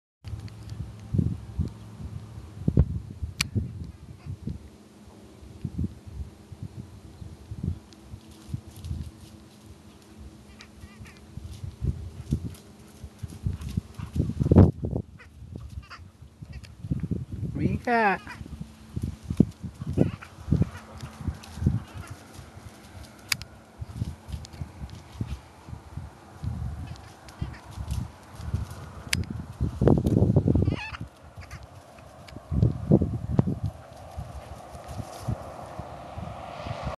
Merlin the puppy!
My buddy, Demian let me "puppy-sit' for a week with "Merlin" (His Puppy)  What a cutie!